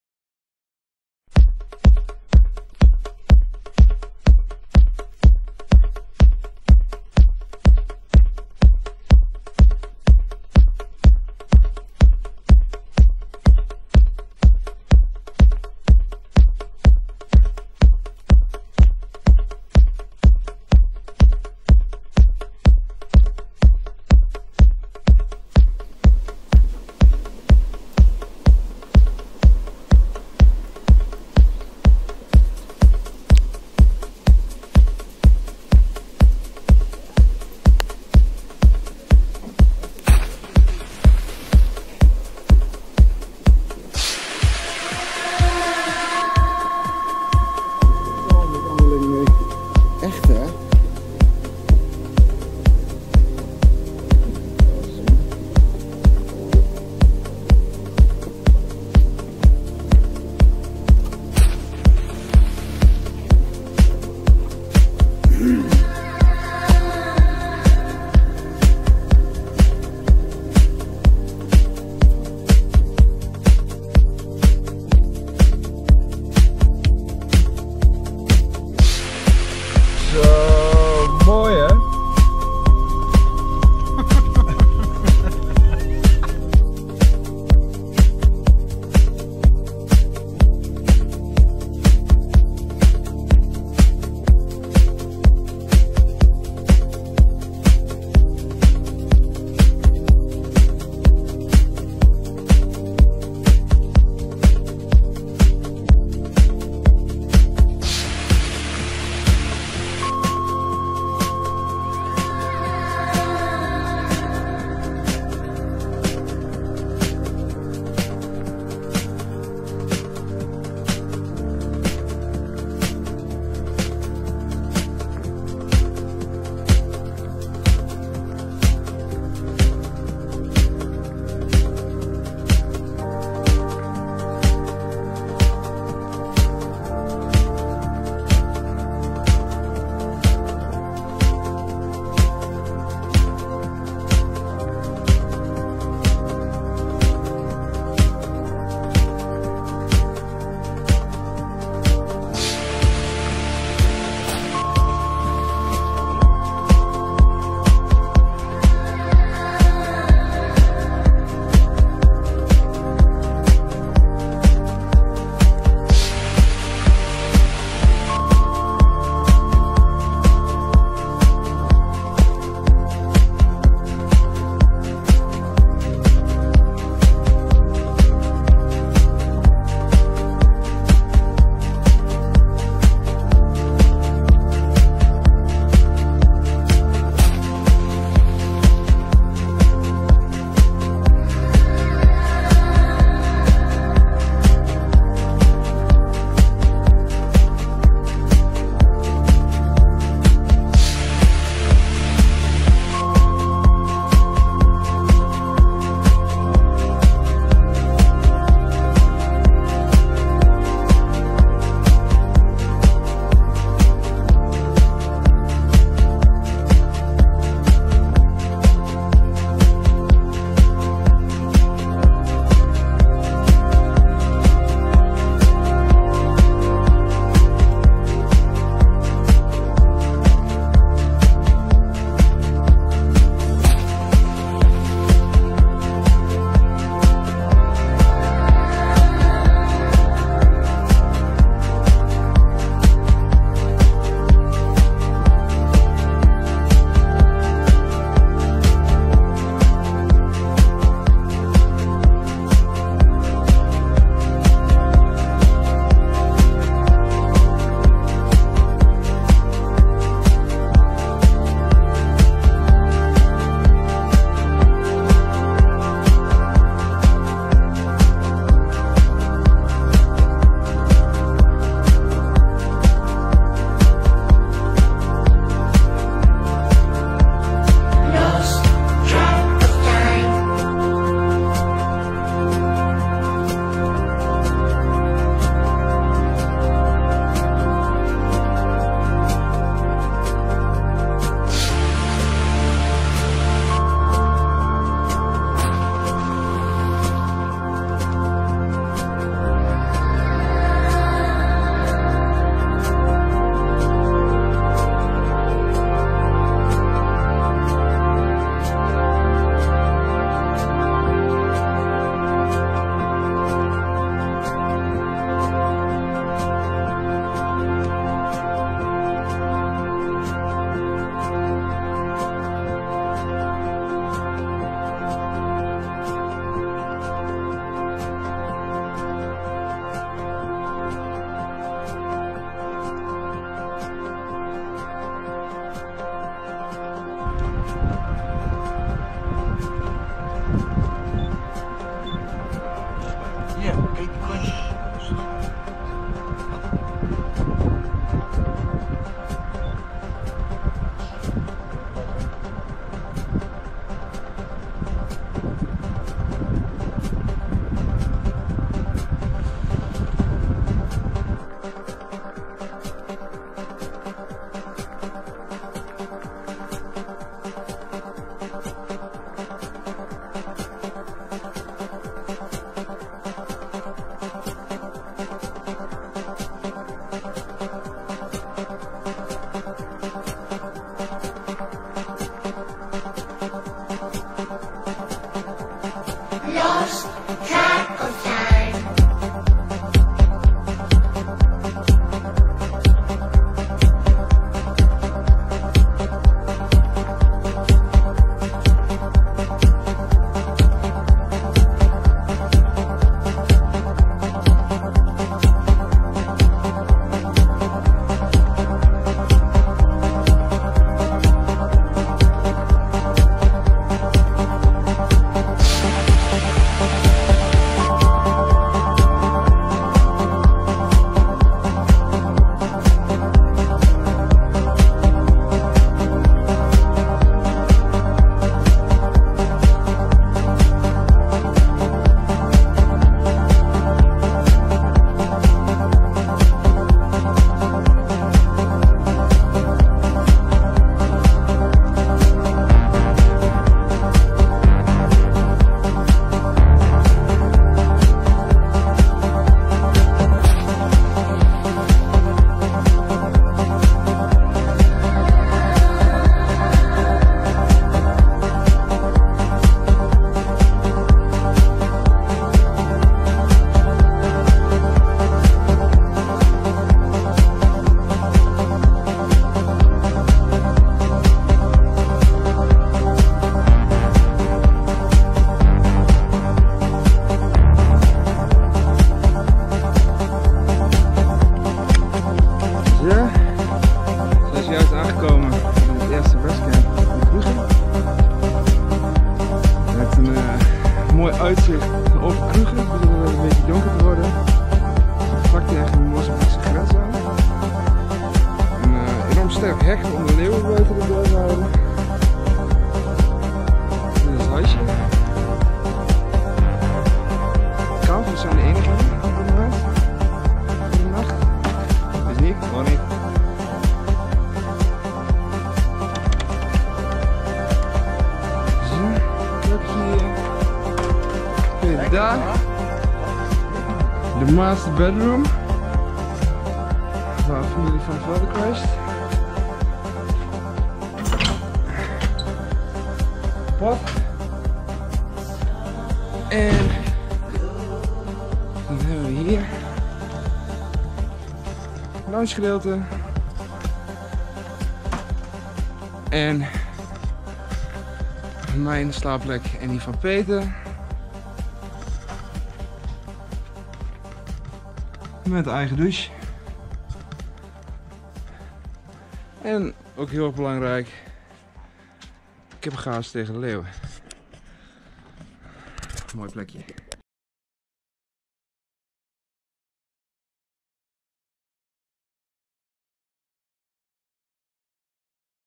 Panorama route to Pafuri Kruger National Park
We flew from Amsterdam to Johannesburg and drove to Gecko Lodge, Hazyview. After a good night we were ready to start our adventure! We took a misty drive through the Panorama route and stayed at Blyde Canyon A Forever Resort for one night. After that through the Phalaborwa gate into the Kruger National Park. On our way to Pafuri border camp we saw tsessebe and a few roan antelope!

0:00 Graskopkloof
2:28 Lisbon falls
3:13 Potluck Boskombuis
3:57 Bourke's luck potholes
4:26 Three rondavels
5:33 Kruger
5:43 Elephant
6:40 Impala
6:58 Zebra
7:12 Tsessebe, roan, nyala
7:52 Buffalo
8:02 Mockford guesthouse, Pafuri border camp